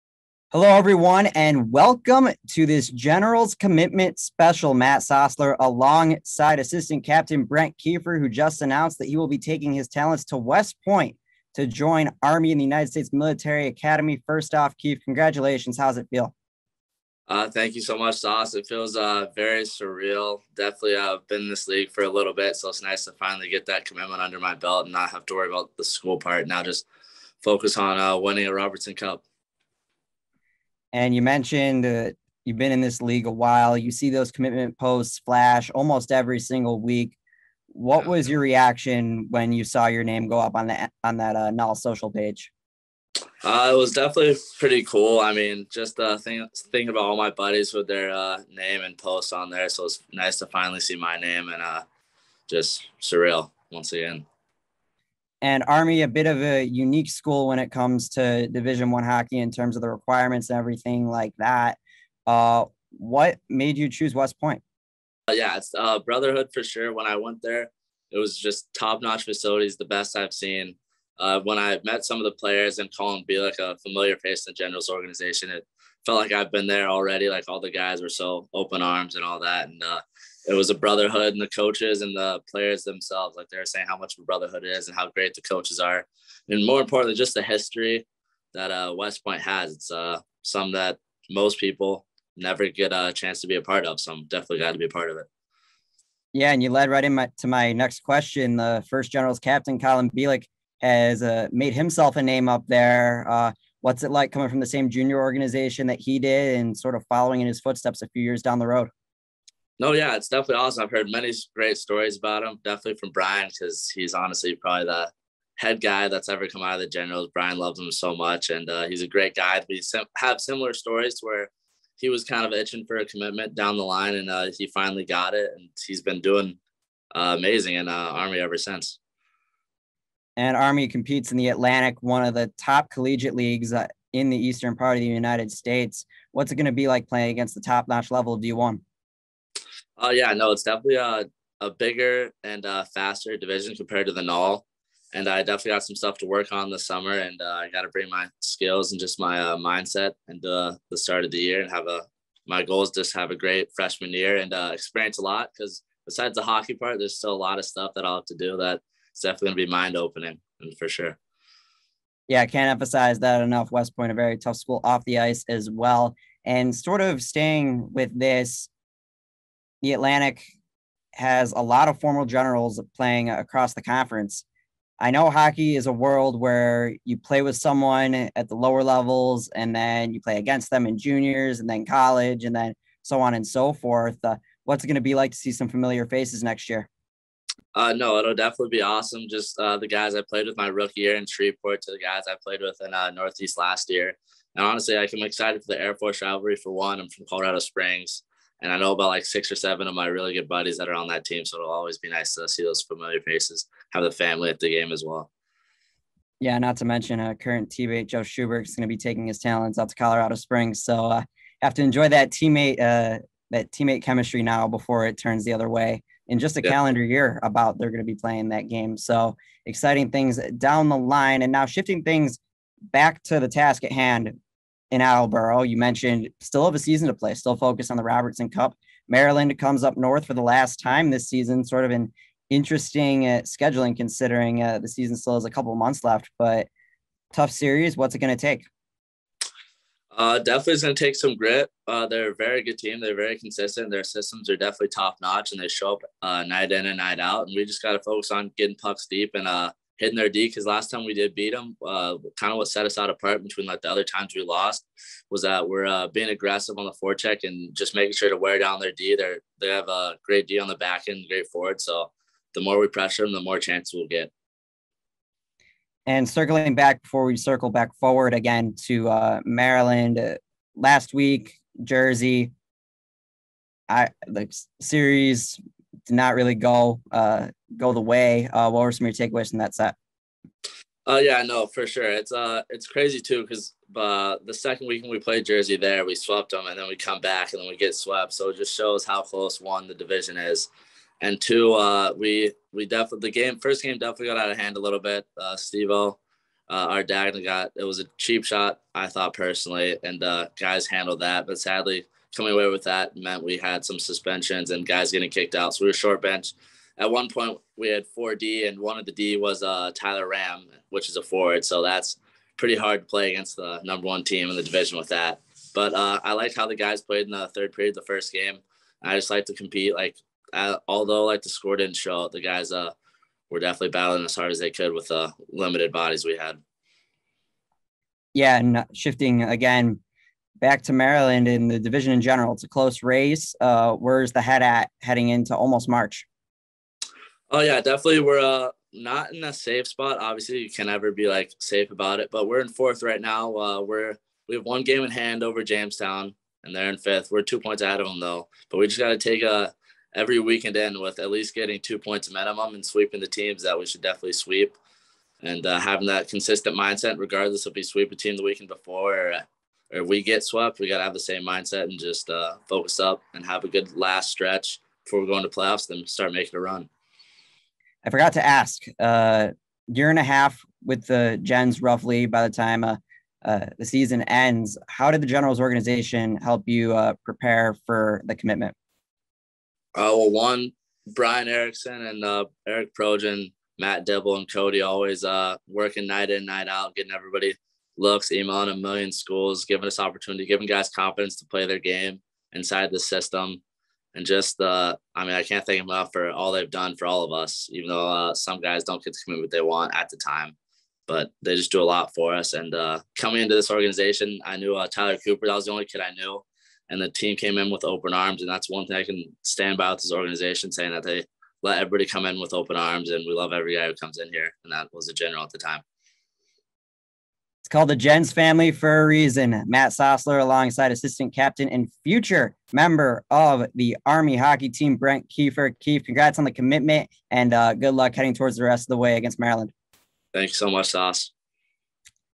Generals Commitment Special: Brent Keefer
Matt Sosler catches up with Brent Keefer following his commitment to Army.